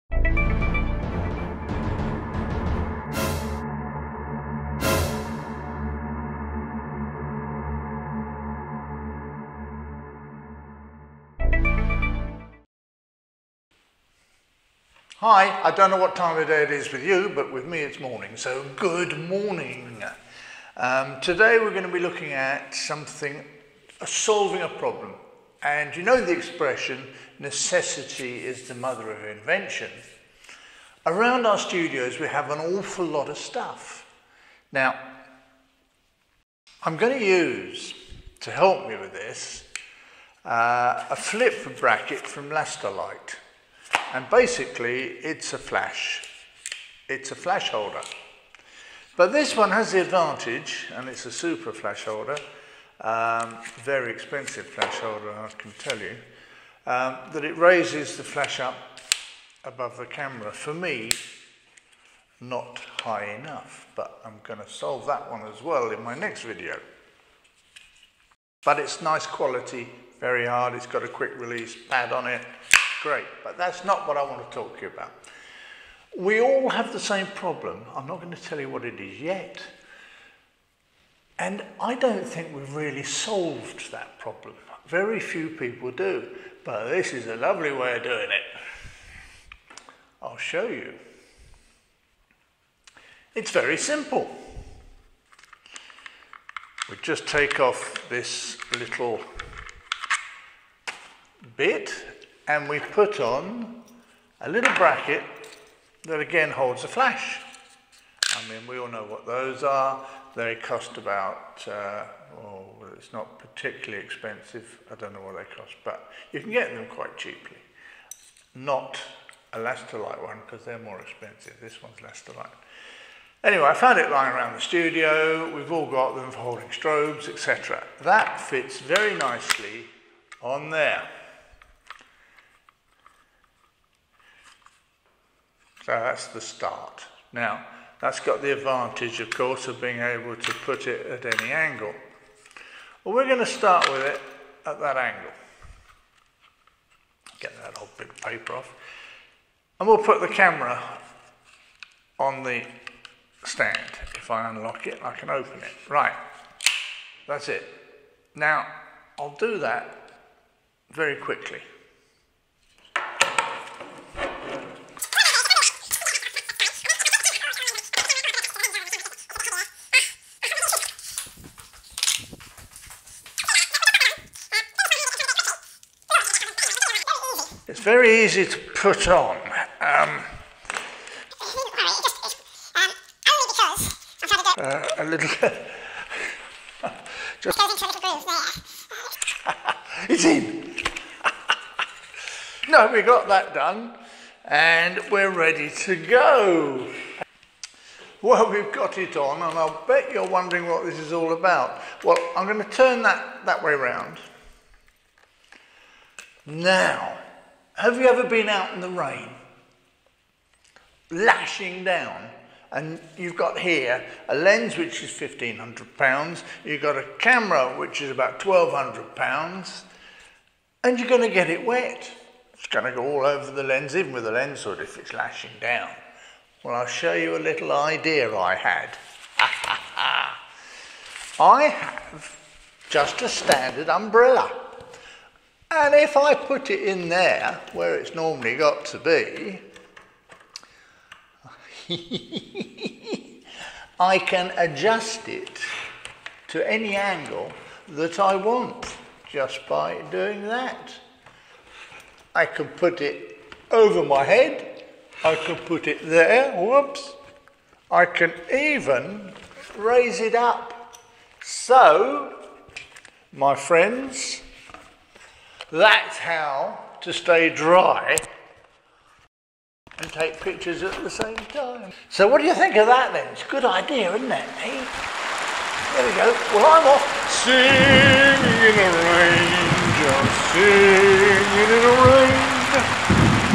photographers problem
A humourus look at solving one of our biggest problems when working outside. Protecting camaers and lenses is very important to the beginner and amateur, a little less so for the pro as it's generally expected that their gear will suffer a lot more from wearand tear. Protecting the camera and lens from bad weather is in fact pretty simple just by using a flash bracket and a photographic umbrella, usually used for studio lighting.
This simple idea will allow you to use your cameras and best lenses , where your friends will not even take their's out of the camera bag. You'll be singing in the rain with this one :)